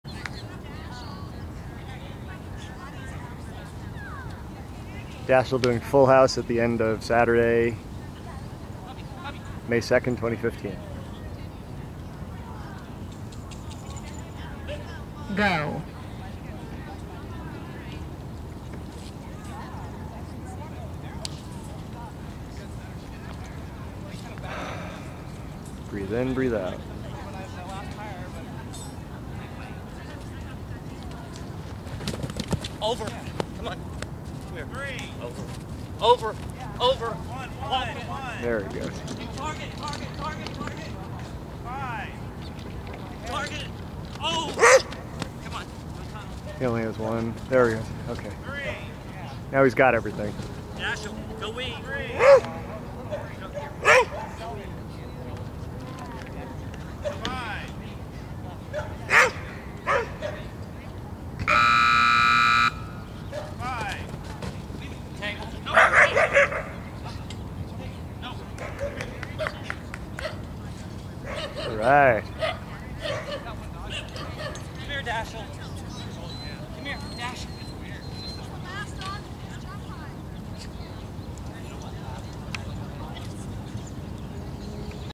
150502 Dashiell FullHouse C
Dashiell and Doug at the West Los Angeles Obedience Training Club's 12th Annual CPE agility trial.